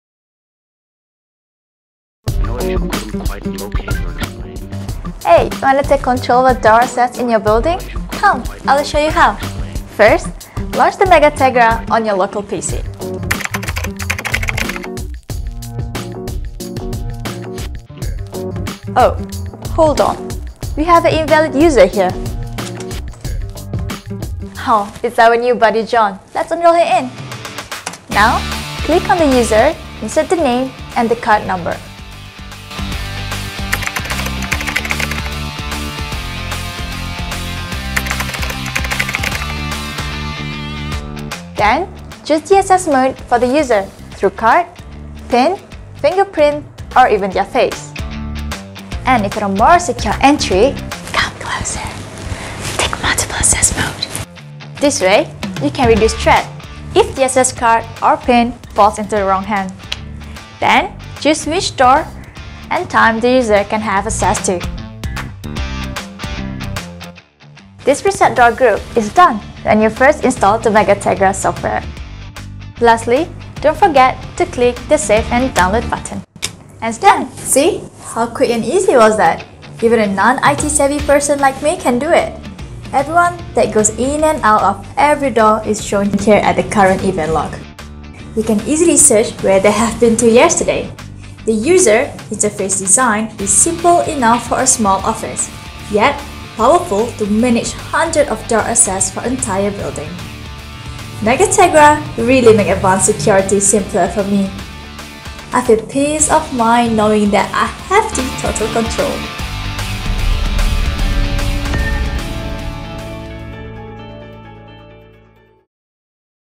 Door Access Solution
Most of access control software in the market is not as user friendly as it should be. Because even though security team need to have an advanced software, the simplicity is necessary as it provides better software navigation especially towards the non IT-savvies.
Simple things like enrolling or deleting a user, assigning different type of access like door for example in this video. When an invalid user is spotted, you can easily edit them and do other things with just a click away. Watch the whole video to learn more.